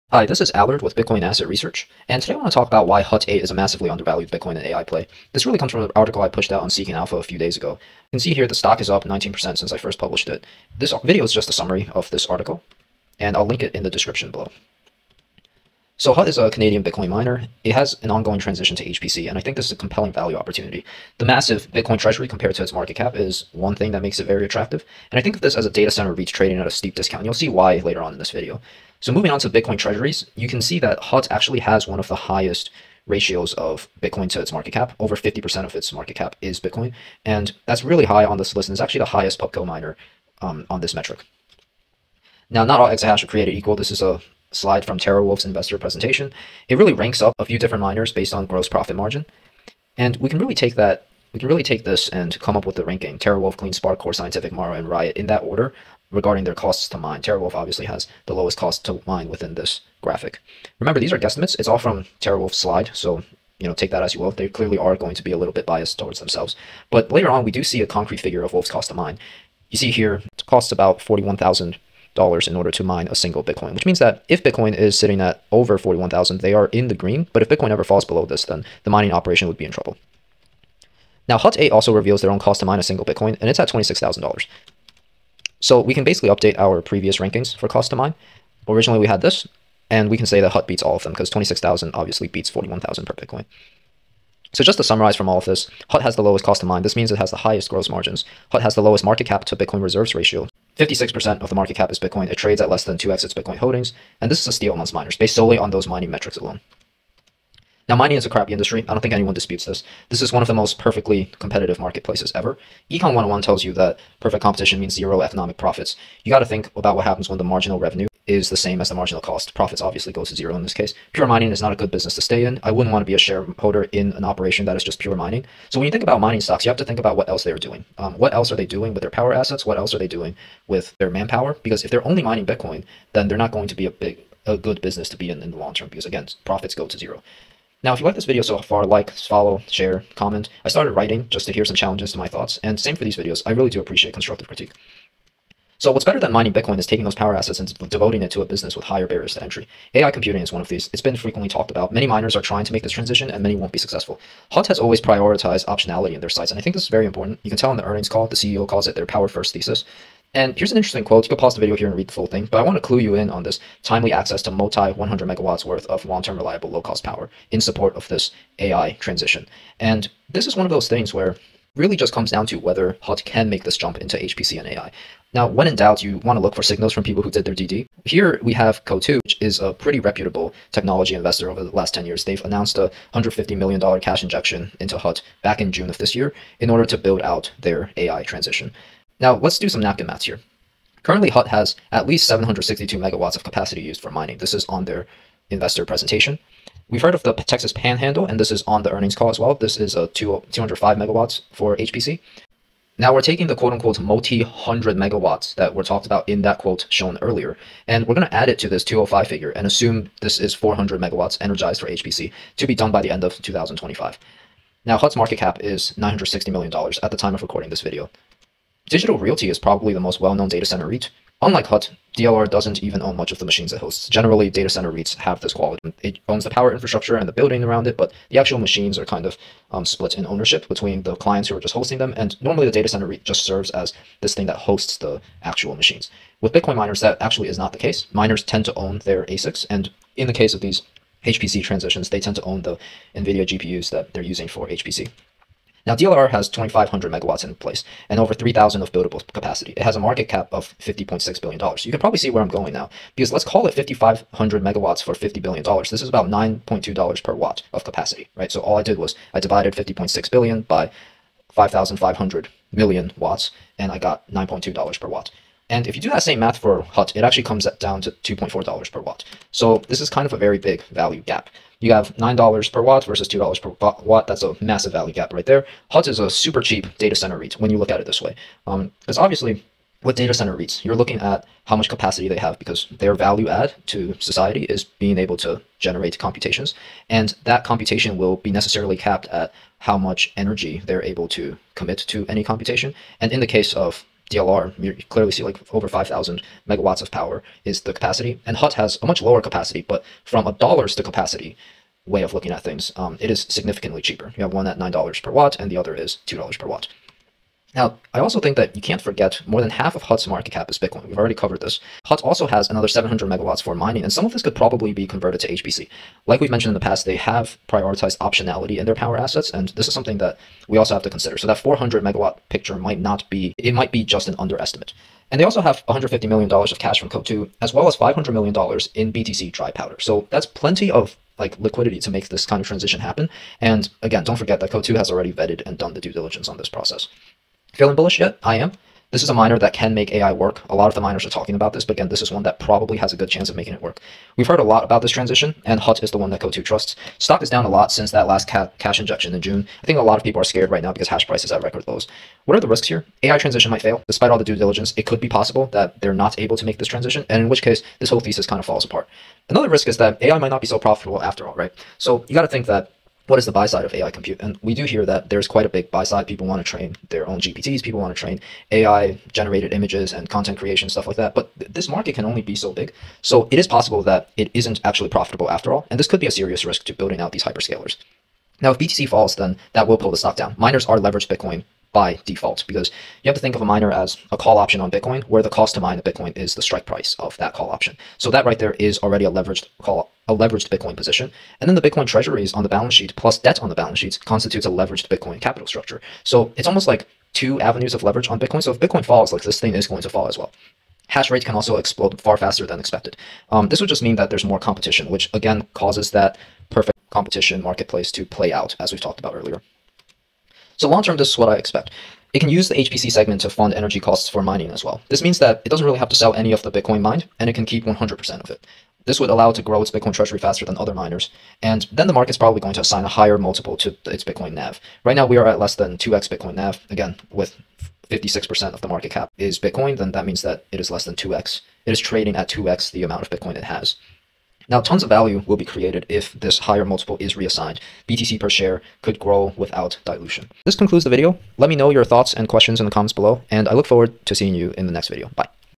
Why Hut 8 Is A Massively Undervalued AI and Bitcoin Stock
Seeking Alpha Link (see comments)

Hut 8 is a data center REIT trading at a steep discount when compared to normal data center REITs. It's much lower cost to mine and huge bitcoin treasury also add a margin of safety. Coatue's investment is a signal that Hut  8 can likely successfully navigate the transition to HPC.